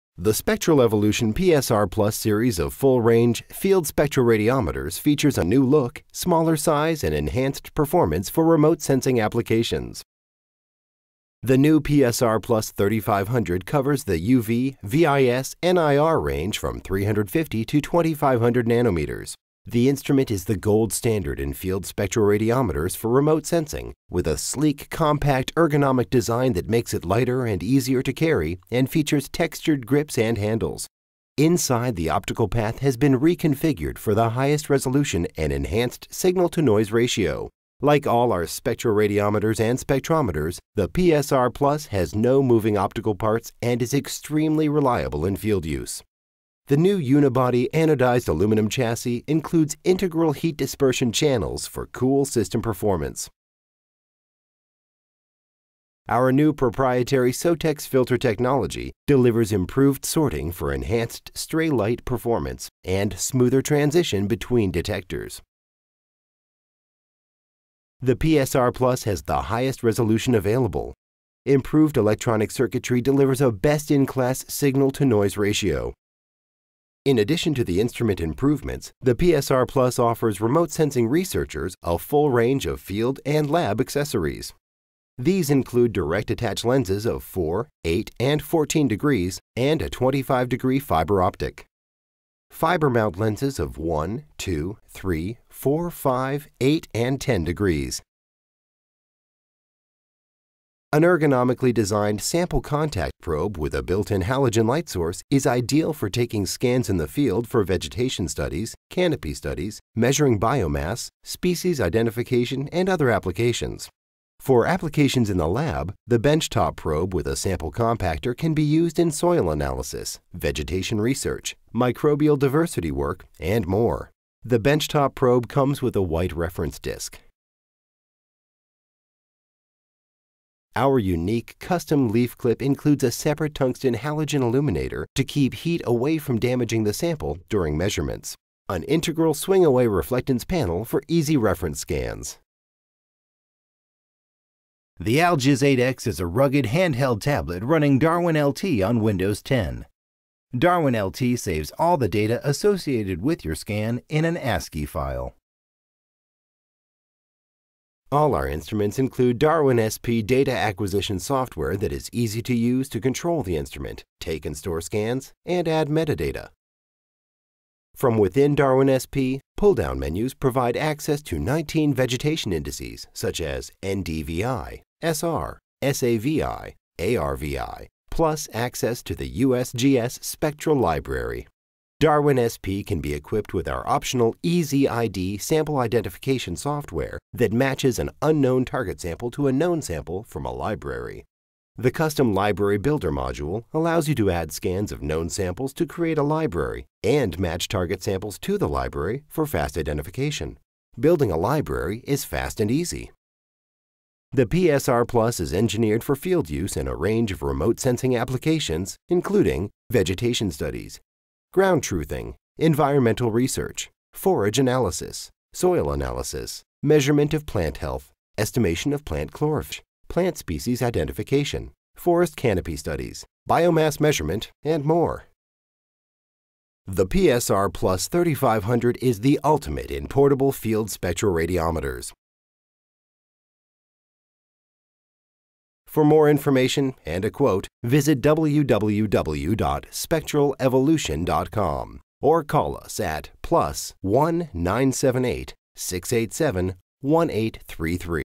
PSR+ field spectroradiometer for remote sensing
PSR+ is a full range UV/VIS/NIR field spectroradiometer with a 350-2500nm spectral range with the highest available resolution and best noise equivalence radiance. A sleek compact design is light, easy to carry, and operate. Enhanced internal design improves optical performance, cool operation, enhanced stray light performance and smoother transition between detectors. Ideal for ground truthing, vegetation studies, forest canopy research, biomass studies, soil analysis, and more.